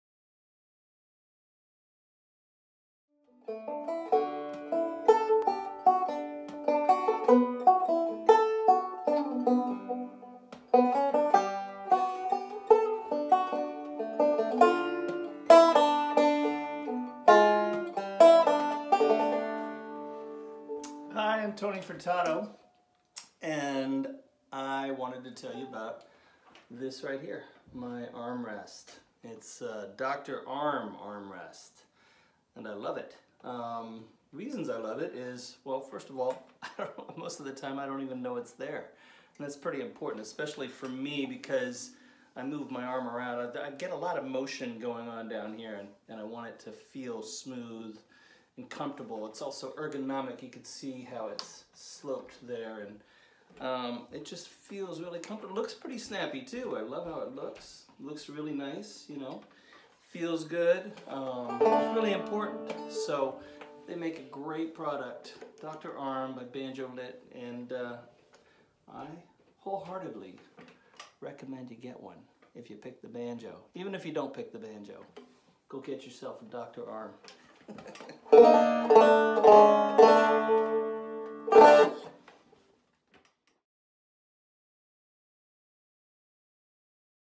Tony Furtado - DR. ARM banjo armrest
“I Love my Dr. Arm Armrest! I know its great because most of the time, I don’t even notice it’s there…because it’s so comfortable! Ever since I’ve installed it, I feel like my right hand is better positioned to really dig in to the banjo, rather than have the banjo digging in to my wrist!”
-Tony Furtado-